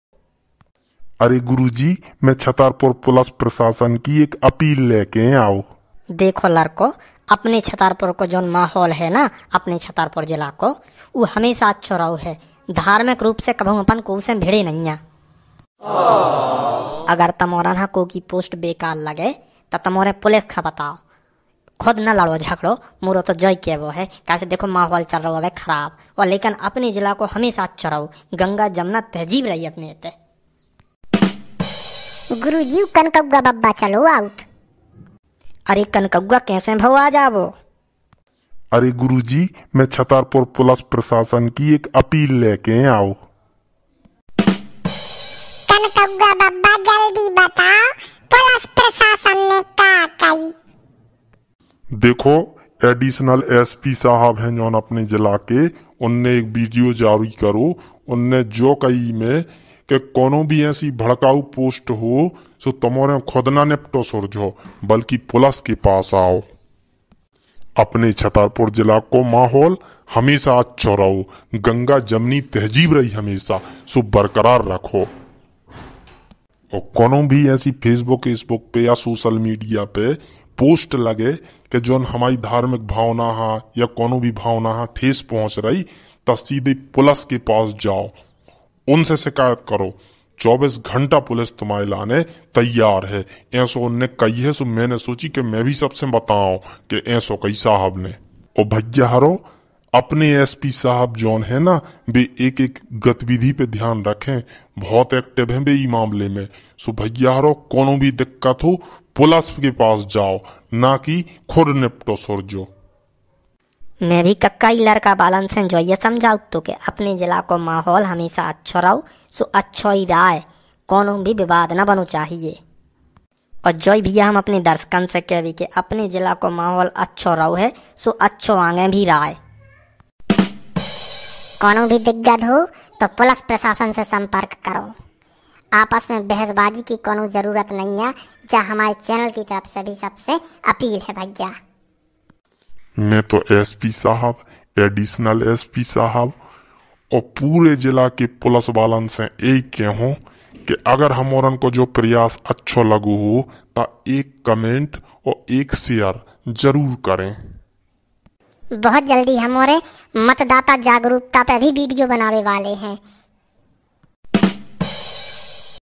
छतरपुर पुलिस प्रशासन का संदेश||Chhatarpur Police Ka Sandesh||Bundeli Mati
सभी दर्शकों से निवेदन हैं कि पुलिस प्रशासन का सहयोग करे
कोई भी समस्या हो पुलिस को सूचित करें
आपकी जानकारी गोपनीय रखी जायेगी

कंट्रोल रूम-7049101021

Bundeli Mati